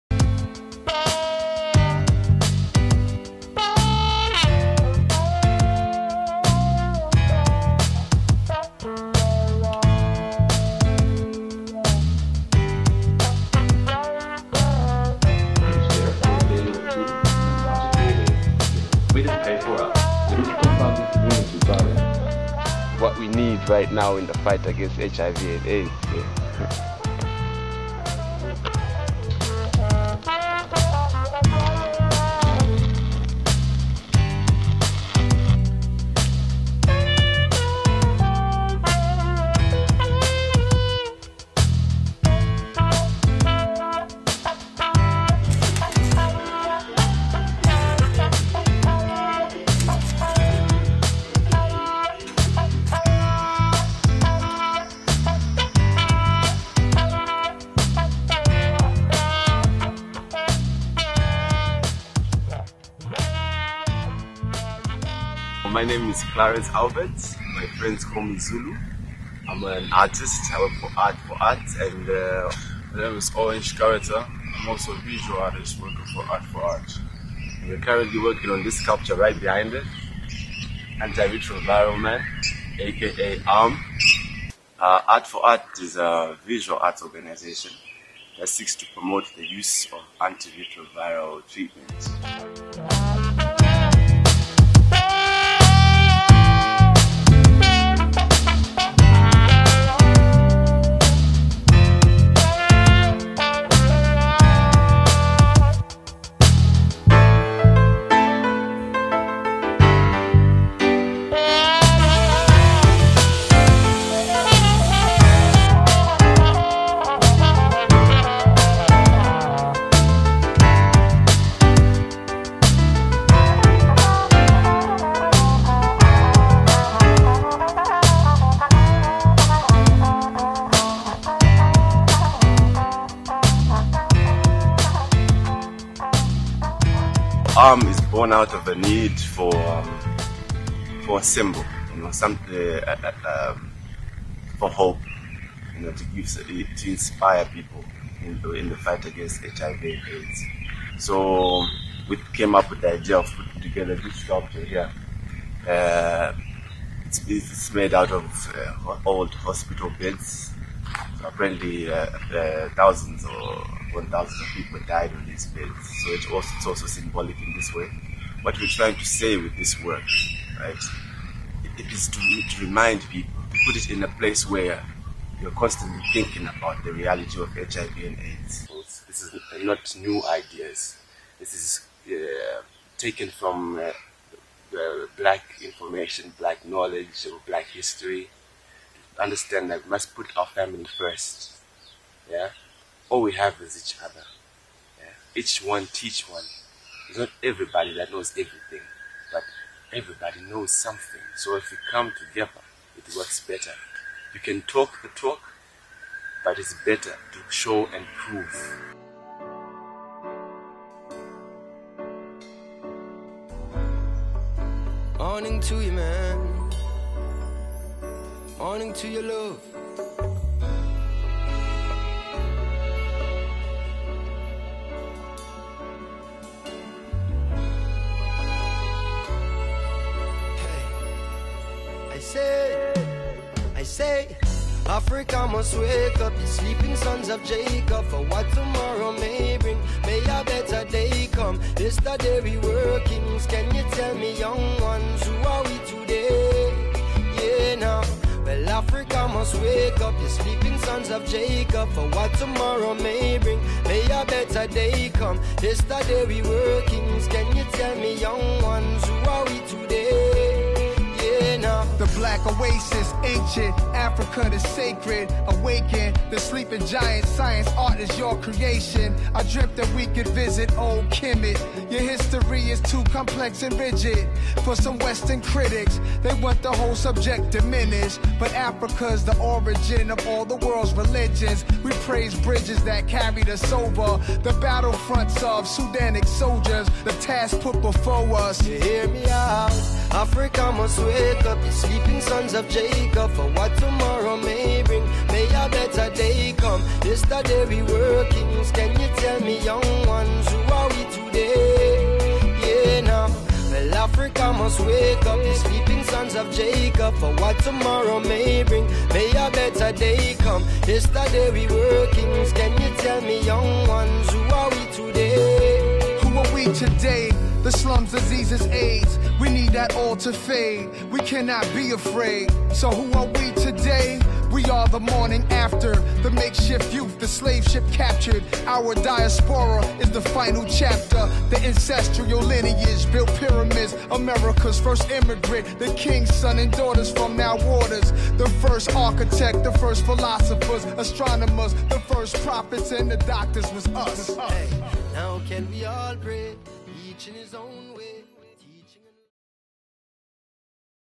World AIDS day 2011 Lusaka Zambia. www. art4art.com
Anti retroviral Man

Too positive to be doubtful
Too optimistic to be fearful
And too determined to be defeated.

Zambia is one of the countries in the Sub Saharan region hardest hit by the AIDS pandemic. The last Zambian Demographic Health Survey which was conducted in 2007, estimated the adult HIV prevalence rate in Zambia to be 14%. Since Anti Retroviral Treatment or ART became available in Zambia in 2004, 320,000 Zambians were initiated on this life saving medication. Even though this is a remarkable achievement, retention in treatment and loss to follow up figures paint a less positive picture. Almost a quarter of the people that start taking ART have stopped (defaulted) the medication 24 months after initiation on therapy.

Art4art is a Zambian artist collaborative, which aims at educating people using art as a medium of expression. For the World AIDS day 2011 art4art has designed and build a 7-meter high statue weighing more than 2 tonnes of Anti Retroviral superman. AntiRetroviral Man is Zambias first superhero. He champions Anti Retroviral Treatment

The AntiRetroviral Man statue is made out of old hospice beds on which thousands of people have died of AIDS. It is modelled after Winstone Zulu Zambias late AIDs activist.

We have the ambition to place the AntiRetroviral Man statue on a landmark site in Lusaka, where it will remind people of the importance of living positively and adhere to treatment.

Art4art